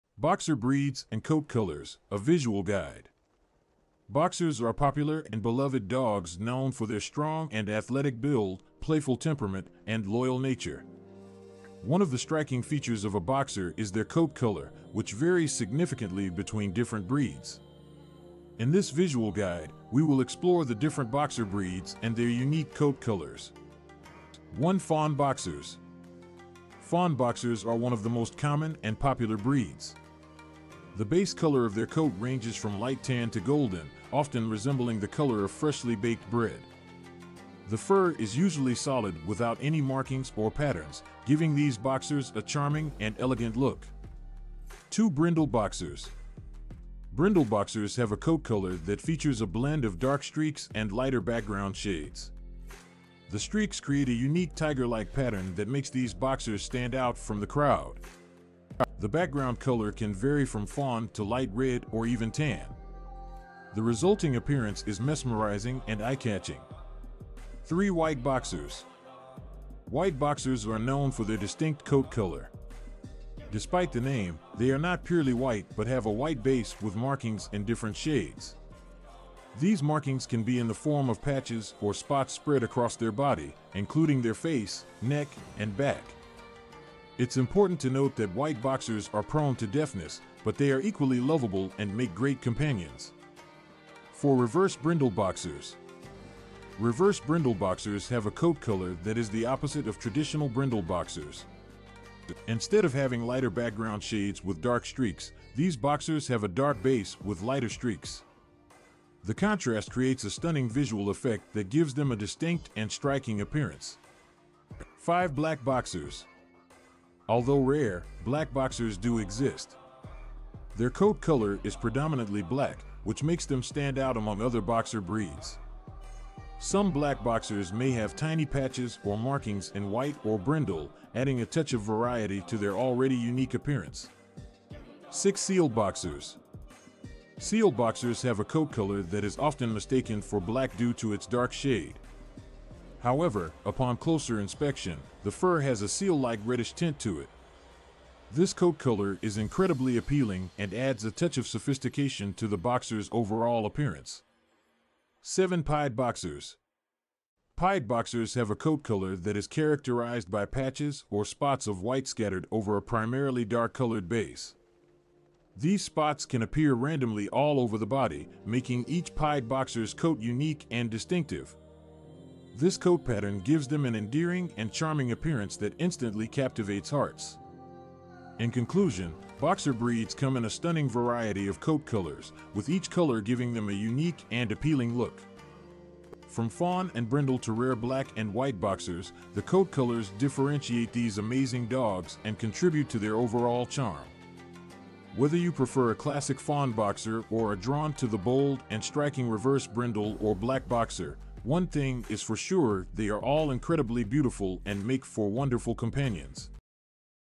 Boxer Breeds and Coat Colors - How Do You Choose the Right One?
Fawn boxers are instantly recognizable for their beautiful coats which range in color from a light tan to a golden hue. These dogs have a short, smooth coat that is often highlighted with a black mask and white markings on their chest, chin, and legs. These friendly and energetic dogs are a popular breed choice for families and make great companions. Fawn boxers are known for their loyal and playful nature, making them a great addition to any home.